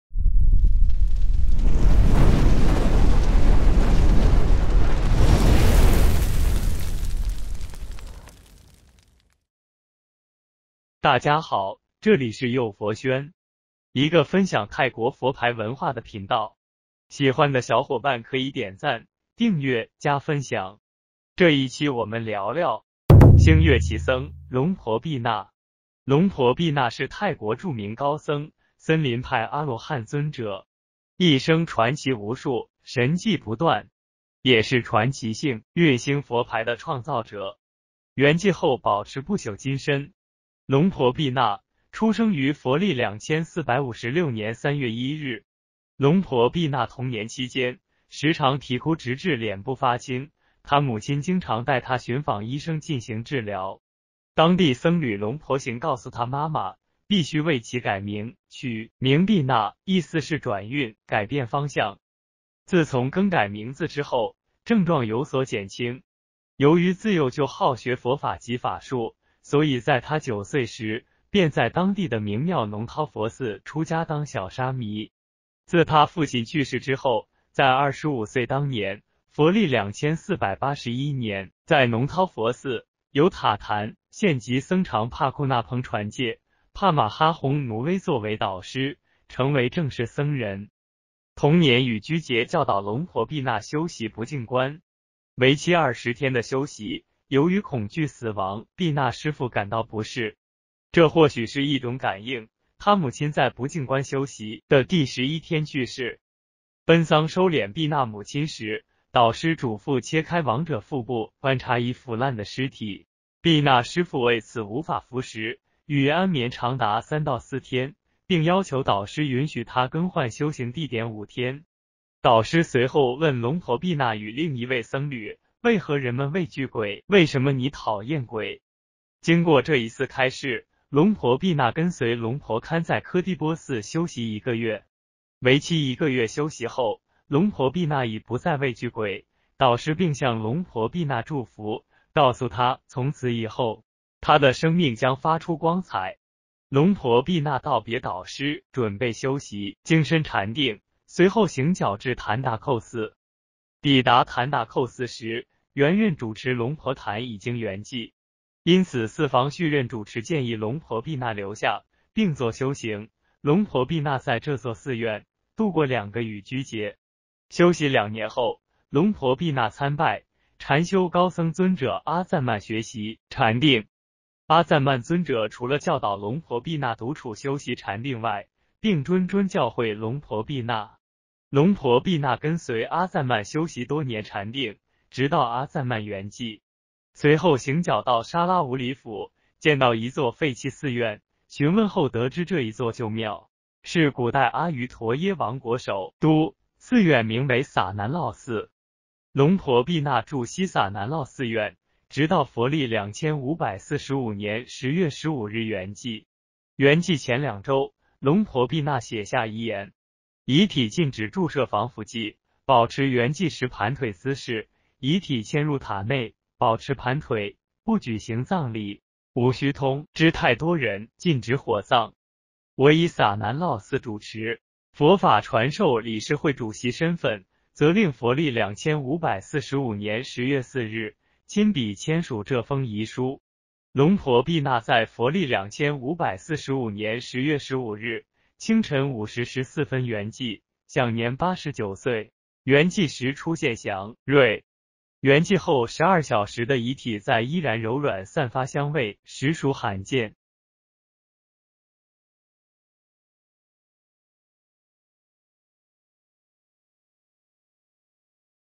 【泰国高僧】为了修行跟随导师嘱咐切开亡者腹部，并观察已腐烂的尸体 | 星月奇僧 「龙婆碧纳」
龙婆碧纳 LUANG PHOR PINAK

龙婆碧纳是泰国著名高僧，森林派阿罗汉尊者。一生传奇无数，神迹不断。也是传奇幸运星佛牌的创造者。圆寂后保持不朽金身。